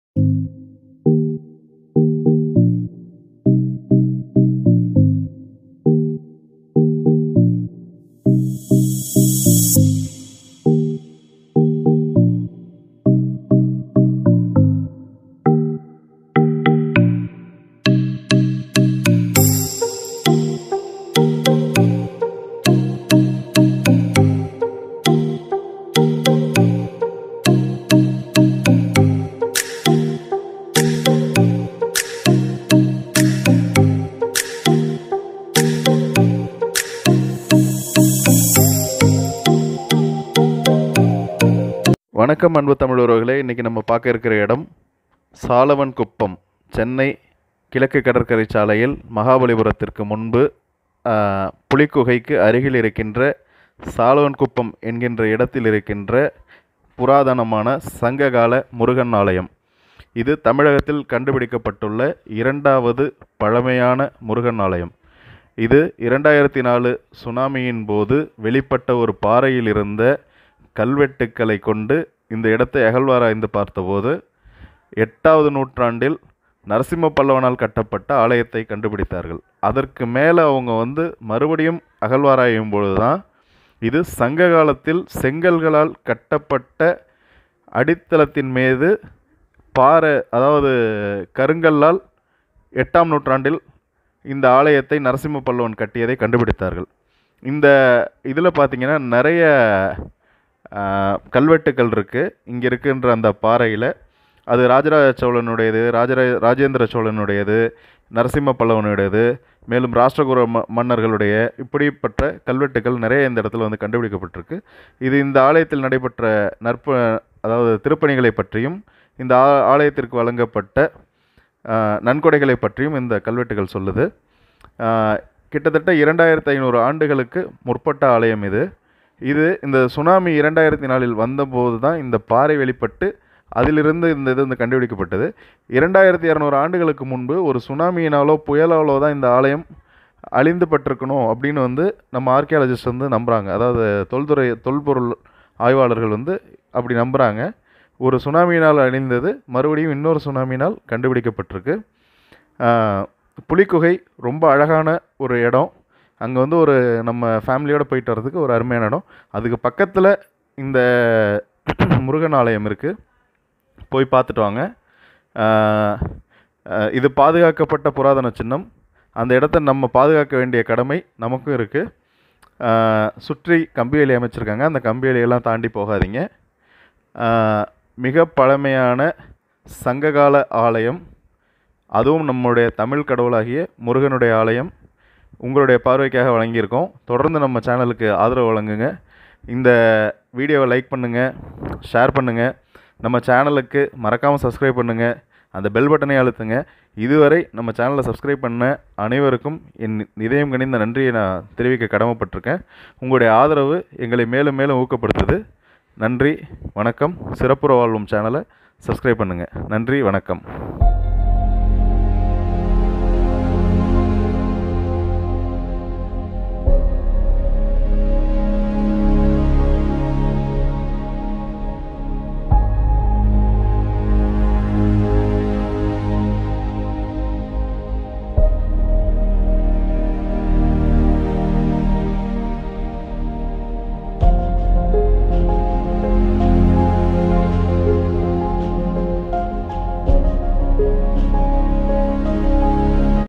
சுனாமியால் வெளிப்பட்ட சங்ககால முருகன் ஆலயம்
சென்னை கிழக்கு கடற்கரை சாலையில் மகாபலிபுரம் அருகில் உள்ள சாலவன் குப்பம் என்ற ஊரில் கண்டறியப்பட்ட சங்ககால முருகன் ஆலயம் 2004ல் ஏற்பட்ட சுனாமி பேரலைகளால் வெளிப்பட்ட பாறையில் காணப்பட்ட கல்வெட்டுக்களின் மூலம் தொல்லியல் துறையால் அகல்வாராய்ந்து கண்டுபிடிக்கபட்டது
2500 ஆண்டுகளுக்கு முற்பட்ட ஆலயம்